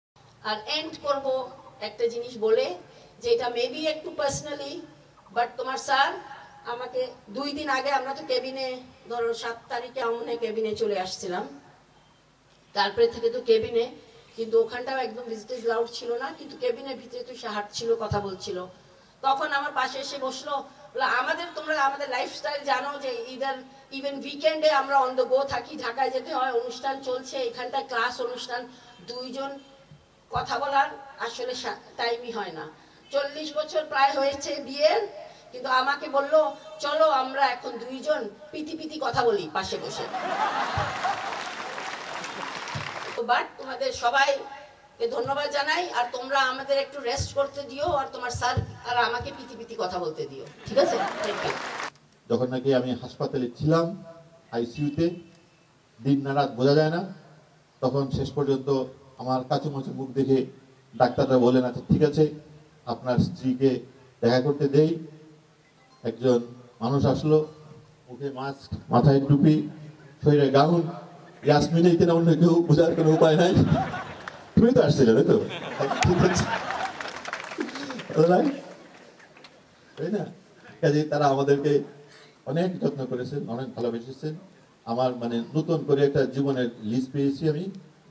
Dr Jafor Iqbal & Dr Yeasmin Haque Funny Speech 14 03 18
Dr  Jafor Iqbal & Dr  Yeasmin Haque Funny Speech @ SUST 14 03 18